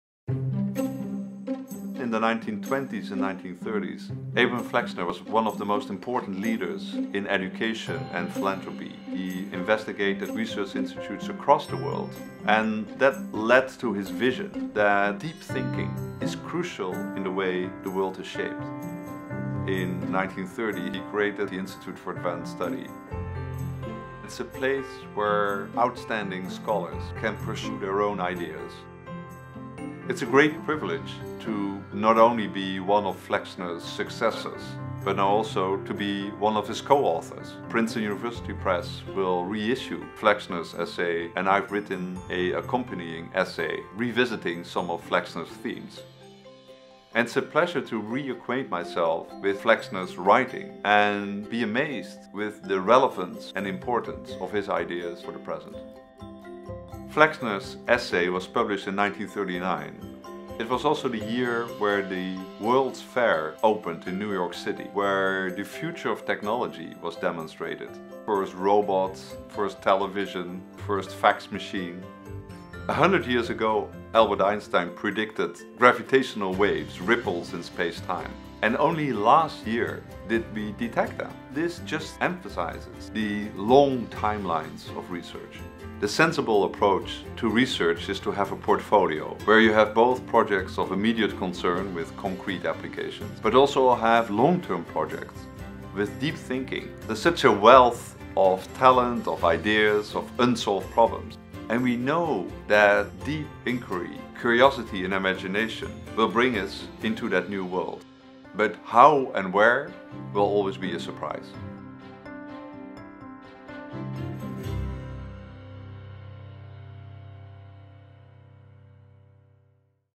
The Usefulness of Useless Knowledge by Abraham Flexner
A forty-year tightening of funding for scientific research has meant that resources are increasingly directed toward applied or practical outcomes, with the intent of creating products of immediate value. In such a scenario, it makes sense to focus on the most identifiable and urgent problems, right? Actually, it doesn't. In his classic essay "The Usefulness of Useless Knowledge," Abraham Flexner, the founding director of the Institute for Advanced Study in Princeton and the man who helped bring Albert Einstein to the United States, describes a great paradox of scientific research. The search for answers to deep questions, motivated solely by curiosity and without concern for applications, often leads not only to the greatest scientific discoveries but also to the most revolutionary technological breakthroughs. In short, no quantum mechanics, no computer chips.

This brief book includes Flexner's timeless 1939 essay alongside a new companion essay by Robbert Dijkgraaf, the Institute's current director, in which he shows that Flexner's defense of the value of "the unobstructed pursuit of useless knowledge" may be even more relevant today than it was in the early twentieth century. Dijkgraaf describes how basic research has led to major transformations in the past century and explains why it is an essential precondition of innovation and the first step in social and cultural change. He makes the case that society can achieve deeper understanding and practical progress today and tomorrow only by truly valuing and substantially funding the curiosity-driven "pursuit of useless knowledge" in both the sciences and the humanities.